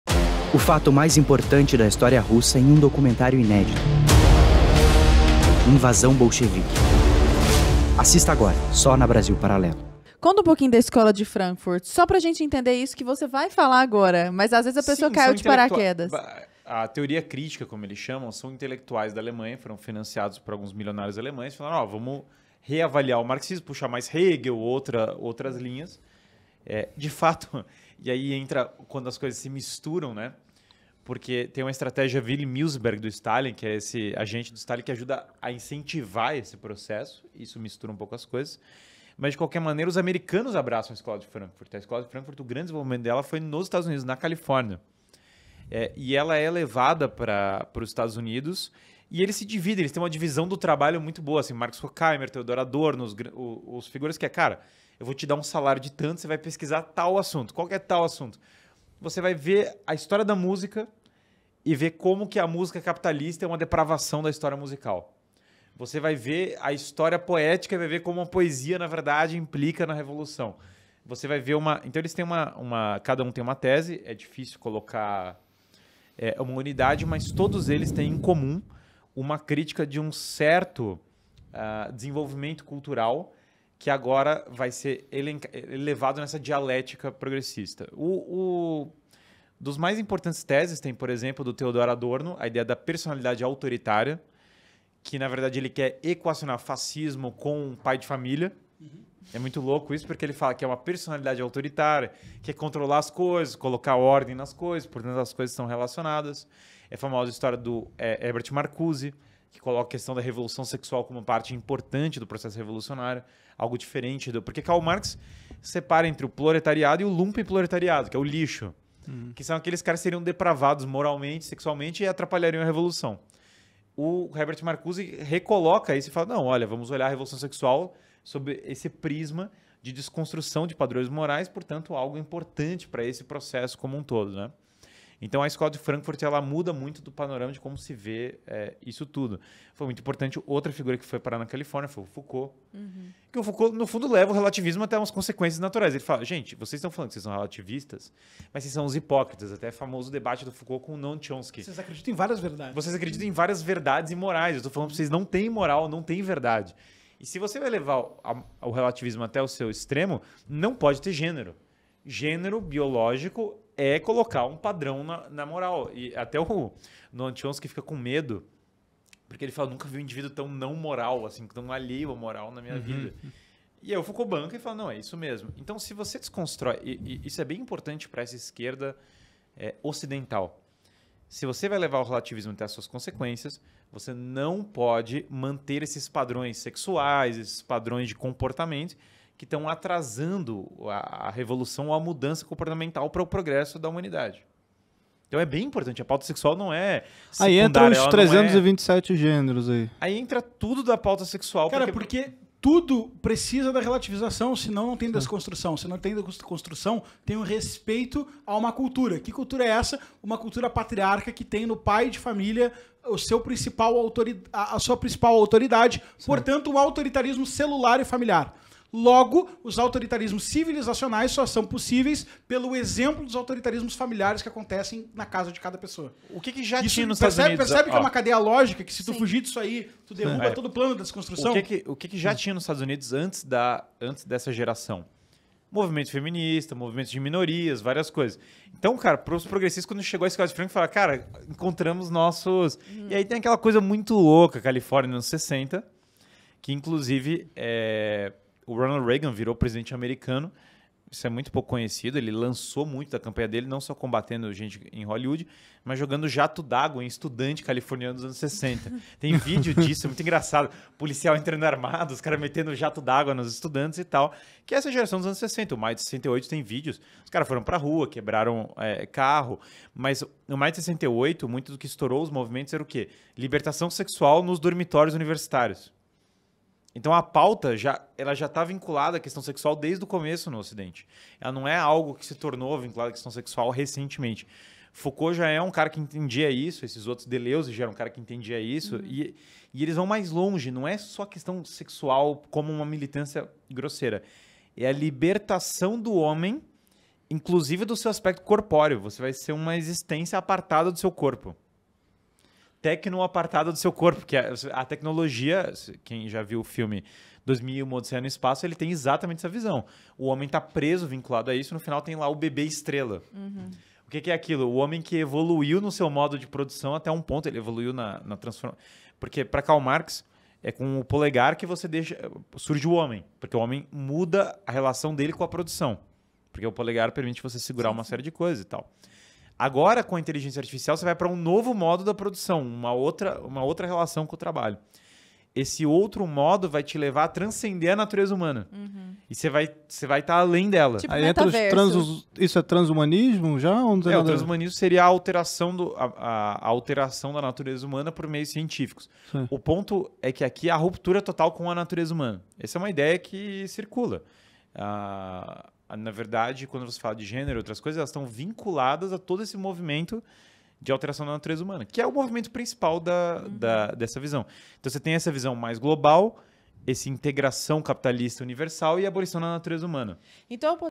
O que nunca te contaram sobre a Escola de Frankfurt
O FATO MAIS IMPORTANTE DA HISTÓRIA RUSSA EM UM DOCUMENTÁRIO INÉDITO
A escola de Frankfurt foi uma escola de pensamento que surgiu sob o nome de Instituto de Pesquisas Sociais na Universidade de Frankfurt, Alemanha. Desde o princípio, foi criada com a finalidade de destruir a cultura ocidental, atacando o capitalismo, a dimensão religiosa da vida e a tradição clássica.

O marxismo original foi adaptado para que influenciasse o Ocidente e mudasse a cultura por meio das ideias e da linguagem, alterando os hábitos das pessoas.

Os principais pensadores da Escola de Frankfurt eram filósofos ou sociólogos marxistas, como:

Max Horkheimer;
Friedrich Pollock;
Theodor Adorno;
Herbert Marcuse;
Erich Fromm;
Jurgen Habermas;
Wilhelm Reich.

As ideias desses teóricos permanecem vivas nos dias de hoje, sobretudo nas universidades e nas grandes mídias de comunicação. A partir do ensino acadêmico e da formação de novos profissionais, as ideias frankfurtianas permeiam a cultura.

Quer saber mais sobre a Escola de Frankfurt? Toque no link e leia nosso artigo